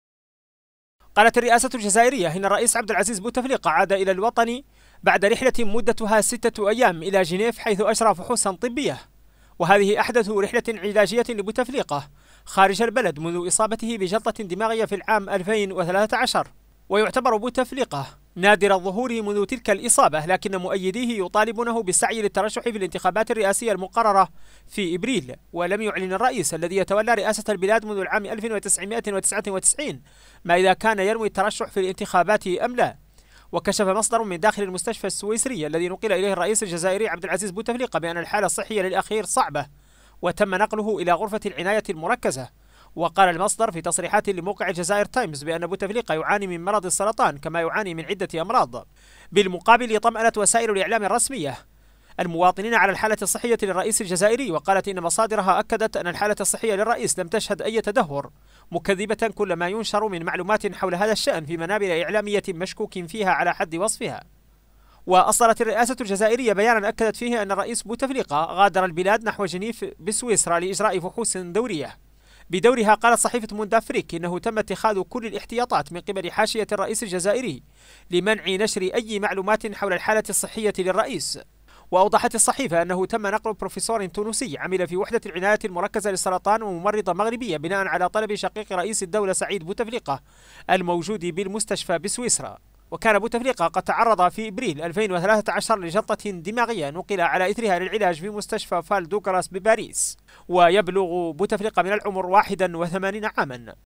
الرئاسة الجزائرية تؤكد عودة بوتفليقة إلى البلاد من رحلة علاجية تقرير الحافظ ولد الغابد.
أكدت الرئاسة الجزائرية عودة رئيس البلاد عبد العزيز بوتفليقة إلى أرض الوطن، قادما من رحلة علاجية قادته إلى جنيف، حيث أجرى فحوصا طبية، تعتبر الأحدث له منذ إصابته بجلطة عام ألفين وثلاثة عشر.
ومنذ إصابته أصبح بوتفليقة قليل الظهور، ورغم وضعيته الصحية، فإن مؤيديه يطالبونه بالترشح لمأمورية رئاسية خامسة.